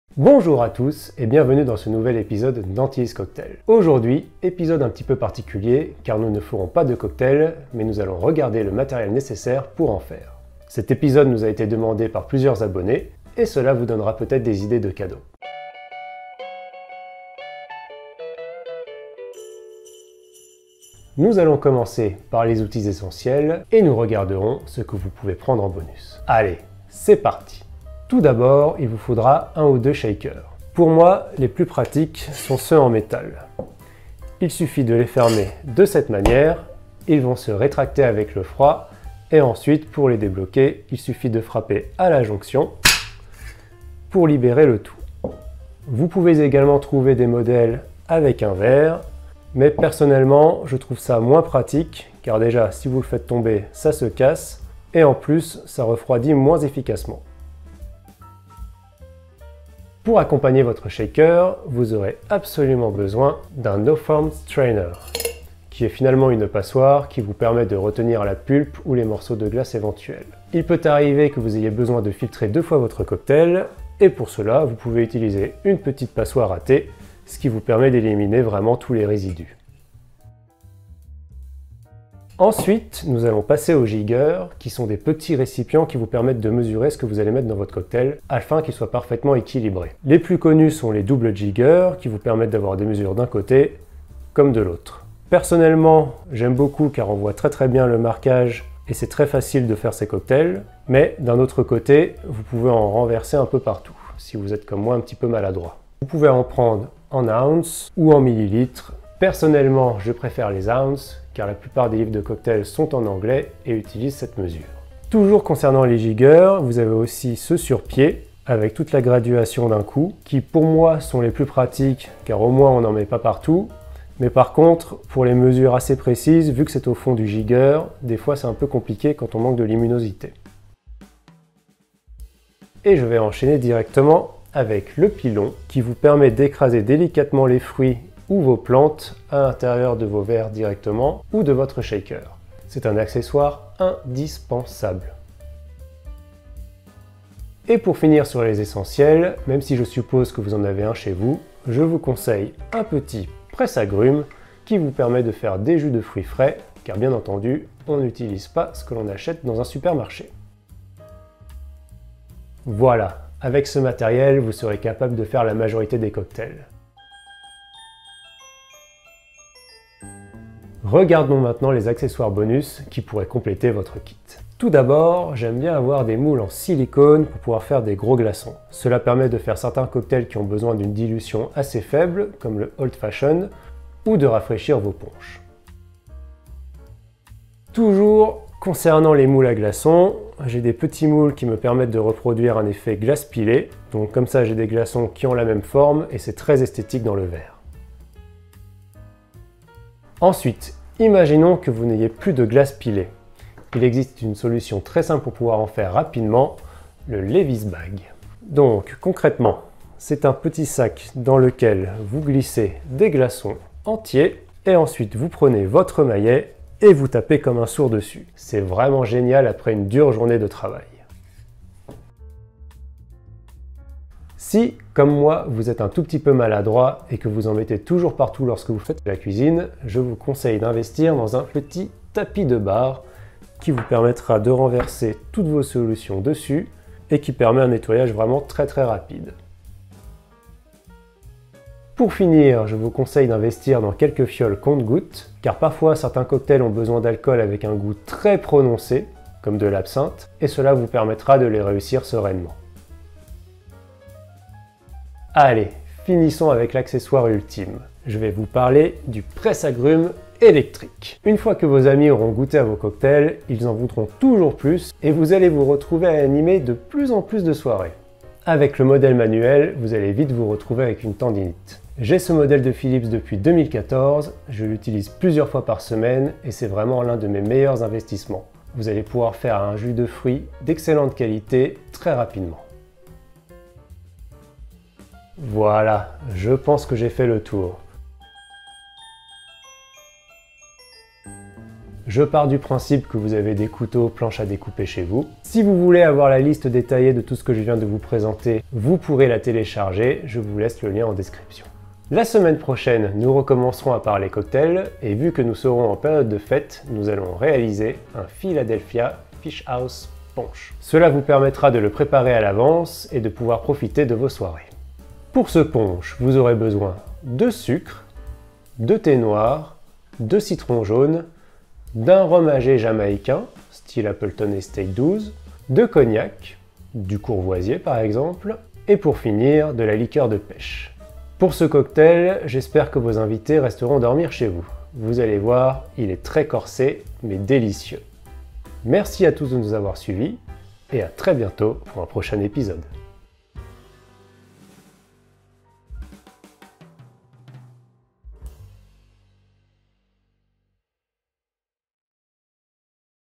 Quel matériel pour faire des bon cocktails ? Parfait pour les cadeaux de dernière minute !
Aujourd'hui, nous allons regarder quel est le matériel nécessaire pour faire des cocktails de bonne qualité, sans se ruiner ;-). Cela vous donnera peut-être des idées de cadeaux pour les fêtes de Noël.

Pour ceux le souhaitant, vous pouvez aussi vous inscrire à notre Newsletter avec un mail tous les vendredis

Chapitres :

0:00 Introduction
0:21 Les essentiels
2:51 Les bonus
5:02 Les ingrédients nécessaires pour le prochain cocktail

Je touche un pourcentage (modeste) des ventes faites sur les liens Amazon ;-) Je ne mets que ce que j'ai aimé :

Faire de la glace pilée :

Méthode douce
Méthode de brute

Antiy's Cocktails

AntiysCocktails Cocktails Matériel Cocktails